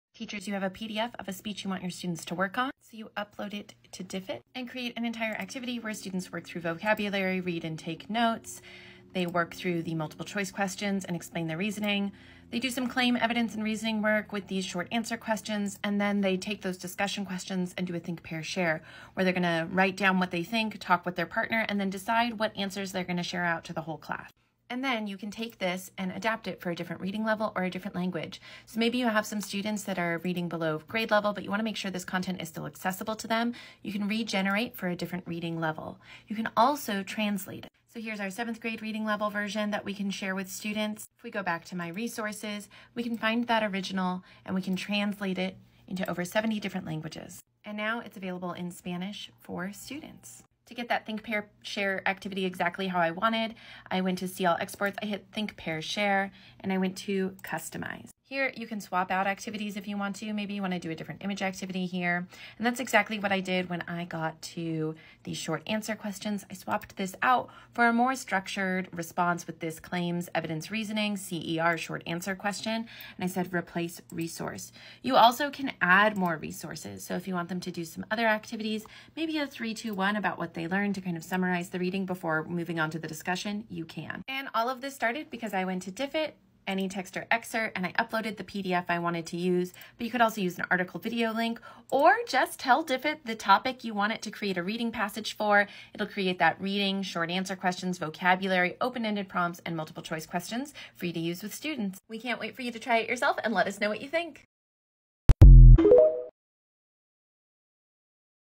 Turn a PDF into a lesson with Diffit!
Teachers use Diffit to instantly get just-right resources for any lesson, saving tons of time and helping all students access grade level content. Head to Diffit.Me to try it yourself!

Any time you adapt or generate a text for the classroom with Diffit it also generates a summary, vocabulary words, and comprehension questions for you to use with your students and in your lesson planning.

Are you an educator looking to share Diffit with your colleagues?